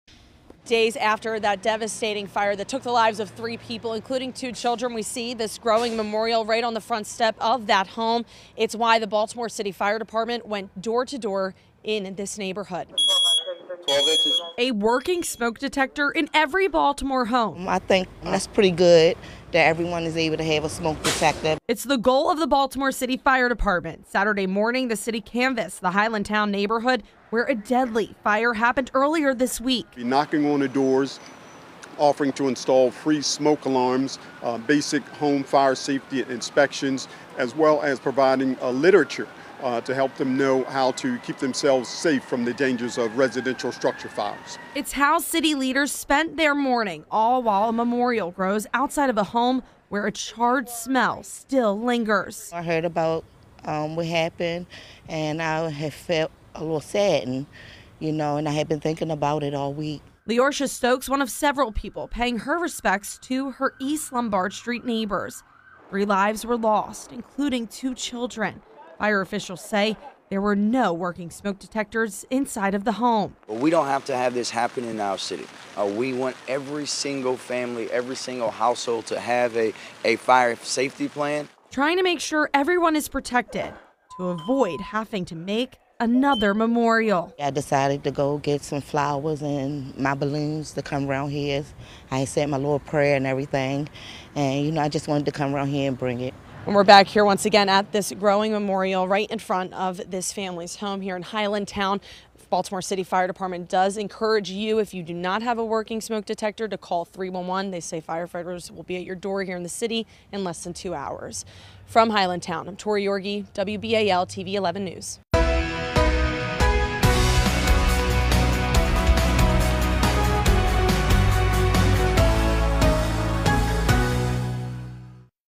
Baltimore Fire Department provides free smoke detectors
The Baltimore City Fire Department hosted a multi-agency Fire Safety Neighborhood Sweep Saturday morning, days after a fatal fire claimed the lives of three people, including two children. City leaders went door to door as part of a fire safety initiative in and around the 3400 block of East Lombard Street in southeast Baltimore.